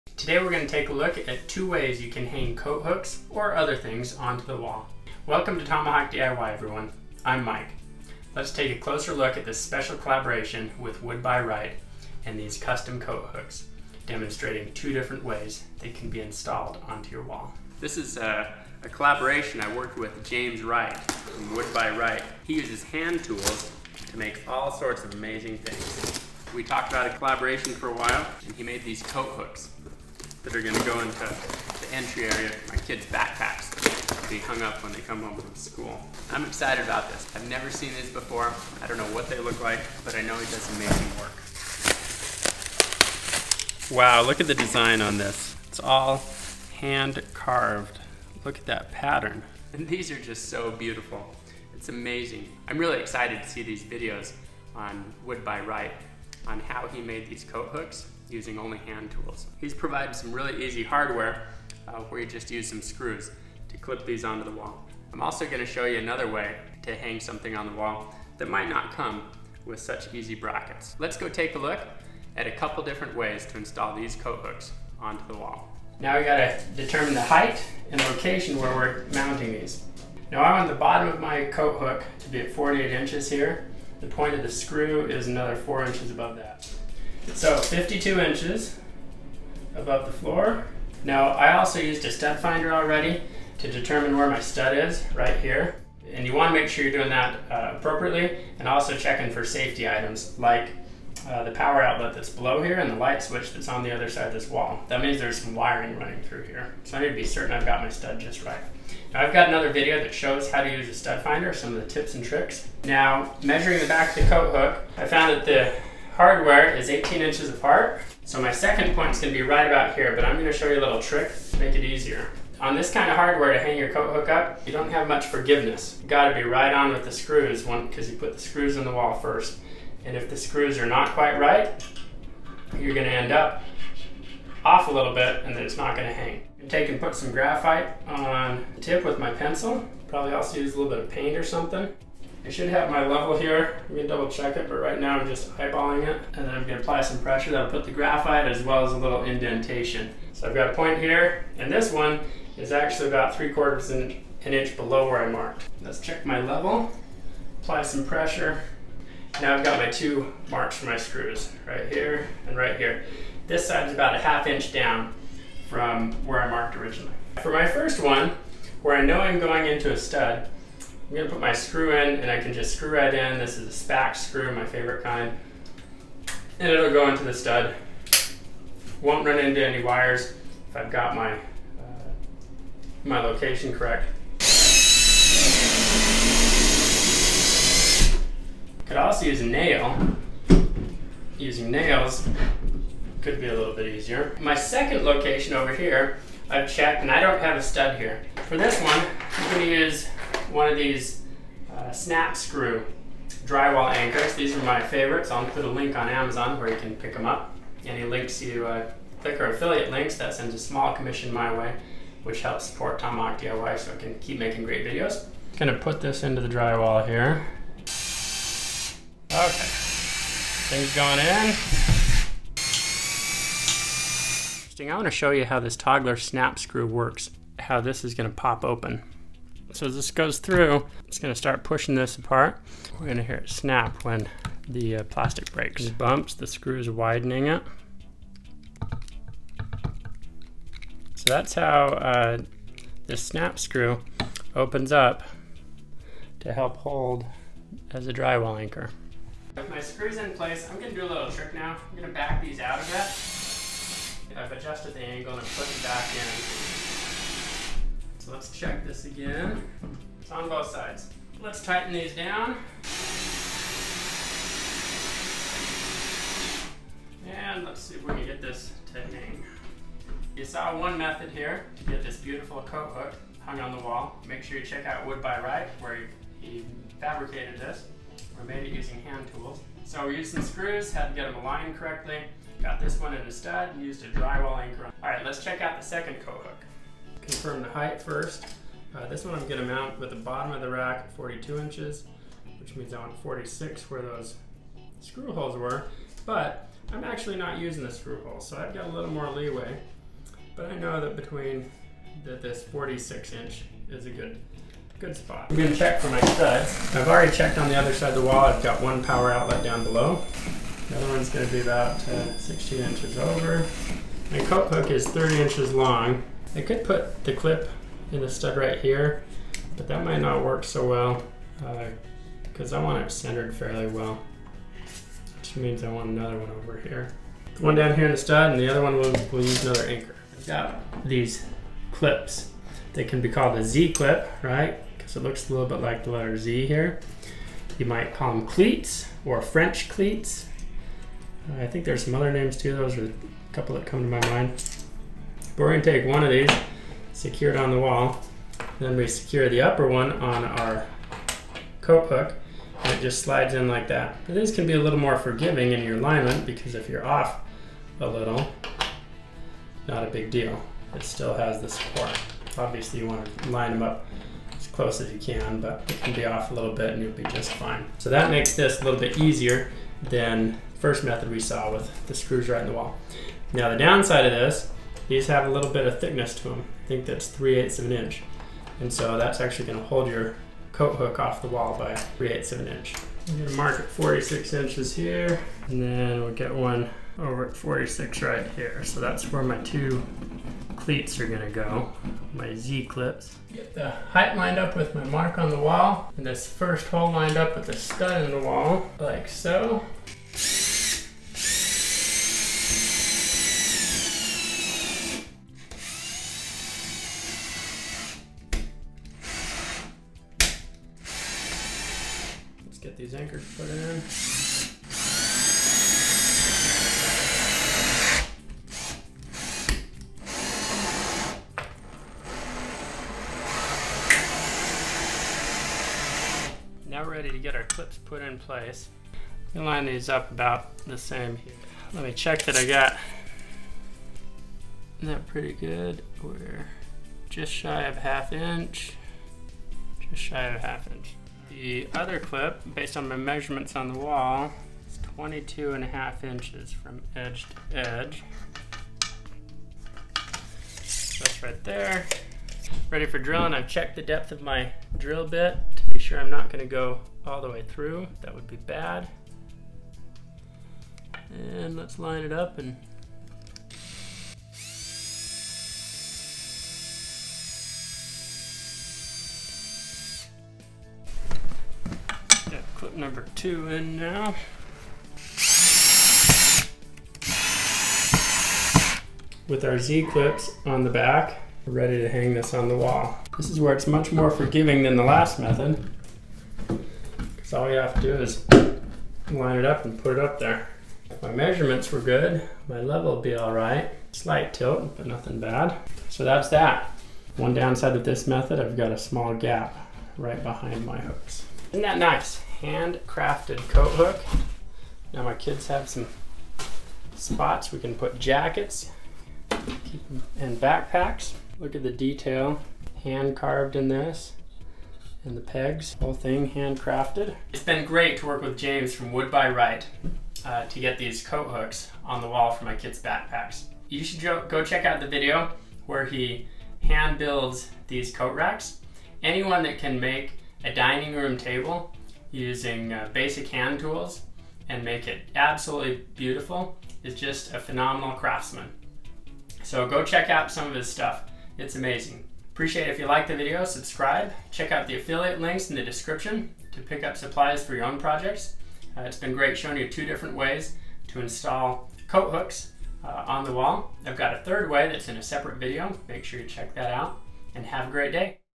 Easy Coat Hook Installation (Hang Things on the Wall)
It isn't hard to hang a coat rack on the wall.  Find out two different ways to get things installed on the wall - this works for coat hooks/racks and also many other large things.
Amazon Canada
Amazon UK
Amazon Canada
Amazon UK

Wall decorations, heavy mirrors, shelving, cabinetry, etc. all can be installed using these techniques.

.317